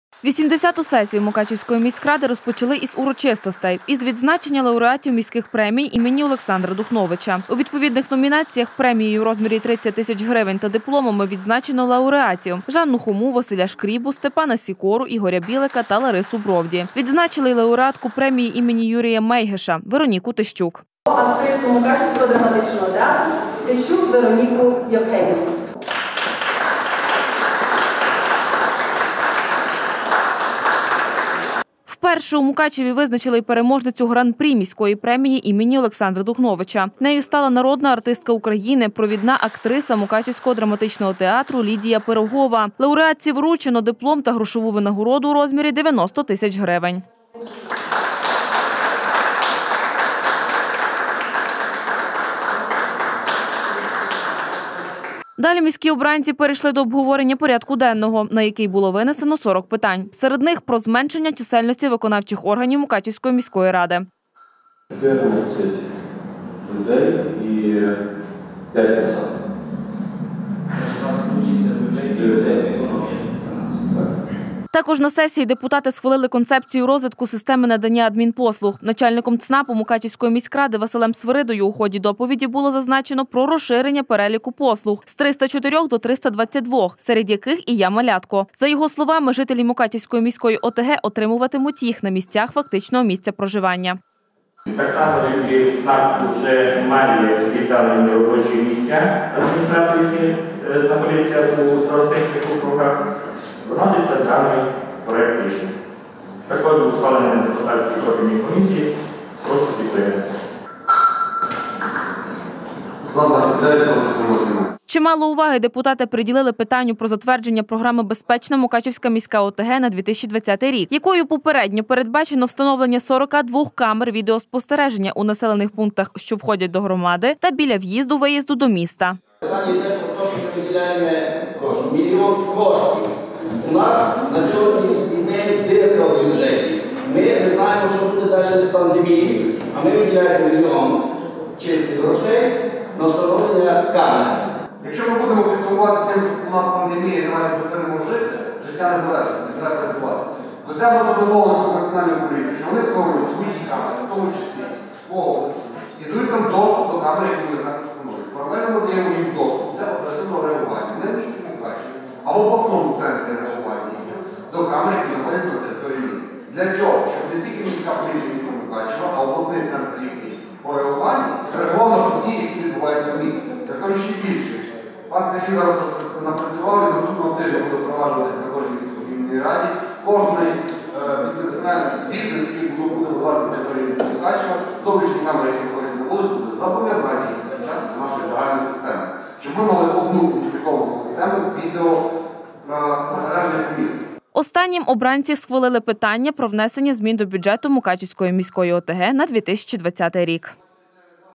Відзначили на сесії
80 сесію Мукачівської міськради традиційно розпочали із урочистостей - із відзначення лауреатів міських премій імені Олександра Духновича. У відповідних номінаціях премією у розмірі 30 тис. гривень та дипломами відзначено лауреатів: Жанну Хому, Василя Шкрібу, Степана Сікору, Ігоря Білика та Ларису Бровді. Відзначили й лауреатку премії ім. Юрія Мейгеша – Вероніку Тищук.
Вперше у Мукачеві визначили і переможницю Гран-прі міської премії ім. О.Духновича. Нею стала народна артистка України, провідна актриса Мукачівського драматичного театру Лідія Пирогова. Лауреатці вручено диплом та грошову винагороду у розмірі 90 тис. гривень.
 Далі міські обранці перейшли до обговорення порядку денного, на який було винесено 40 питань. Серед них – про зменшення чисельності виконавчих органів мукачівської міської ради.Також на сесії депутати схвалили концепцію розвитку системи надання адмінпослуг. Начальником ЦНАПу Мукачівської міськради Василем Свиридою у ході доповіді було зазначено про розширення переліку послуг: з 304 до 322, серед яких і «Я малятко». За його словами, жителі Мукачівської міської ОТГ отримуватимуть їх на місцях фактичного місця проживання.Чимало уваги депутати приділили питанню про затвердження програми «Безпечна Мукачівська міська ОТГ» на 2020 рік, якою попередньо передбачено встановлення 42 камер відеоспостереження у населених пунктах що входять до громади та біля в′їзду-виїзду до міста..Останнім обранці схвалили питання про внесення змін до бюджету Мукачівської міської ОТГ на 2020 рік.